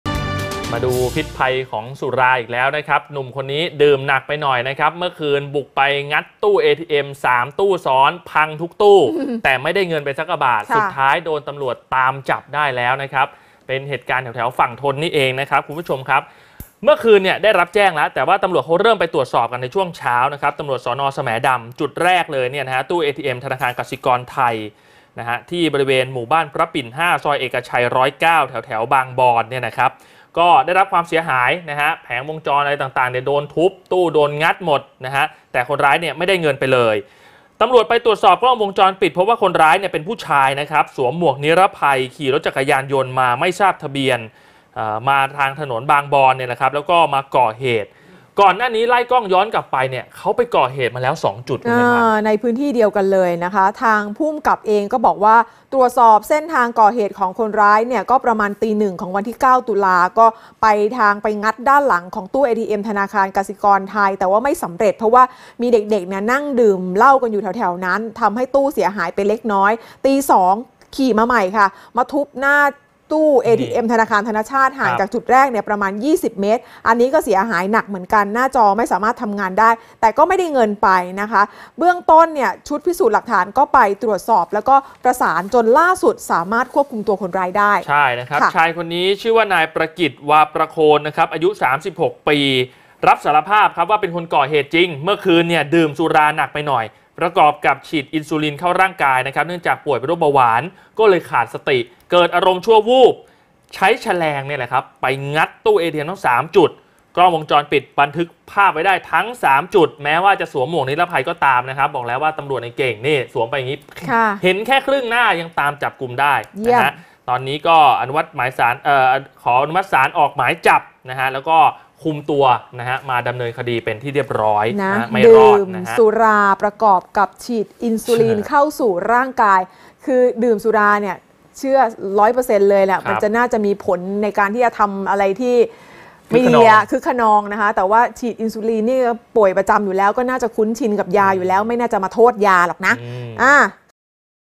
จับแล้วคนร้ายงัดตู้ATM ย่านบางบอน คืนเดียว 3 จุด
รวบคนร้ายงัดตู้ ATM ย่านบางบอนคืนเดียว 3 จุด สารภาพก่อนก่อเหตุดื่มสุรา ทำให้ขาดสติชั่ววูบนำชะแลงไปตะเวนงัดตู้เอทีเอ็ม แต่ไม่ได้เงินไปแม้แต่บาทเดียว